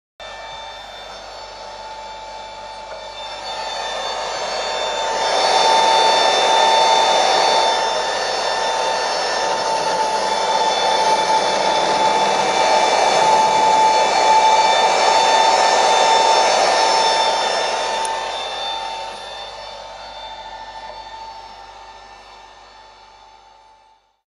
the huge LEGO mobile rocket launcher
the huge LEGO mobile rocket launcher testing video

from instagram id alexandrerossier